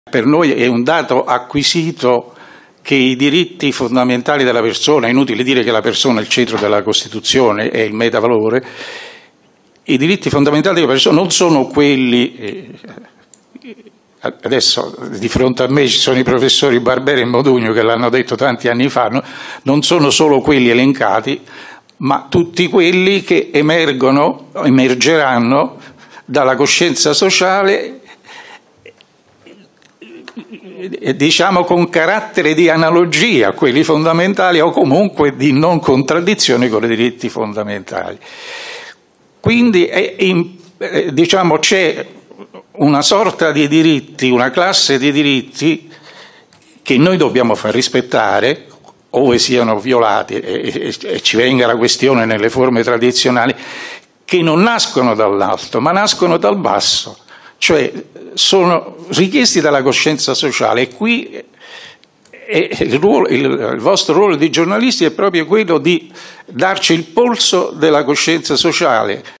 Consulta, il neo presidente Morelli: «Tutelare i diritti richiesti dalla coscienza sociale»
Mario Rosario Morelli è stato eletto come successore di Cartabia

«C'è una classe di diritti che non nasce dall'alto ma dal basso e sono richiesti dalla coscienza sociale per analogia e non contraddizione con i diritti fondamentali», le parole del neo presidente della Corte Costituzionale, Mario Rosario Morelli, durante la conferenza stampa. «I diritti fondamentali della persona non sono solo quelli elencati nella Carta» ha ricordato Morelli. Mario Rosario Morelli è stato eletto anche se non all'unanimità presidente della Corte Costituzionale. Il più anziano in carica, il mandato scade a dicembre. Morelli è stato vice presidente della Consulta durante la presidenza di Cartabia. ( Ansa - CorriereTv ). Guarda il video su Corriere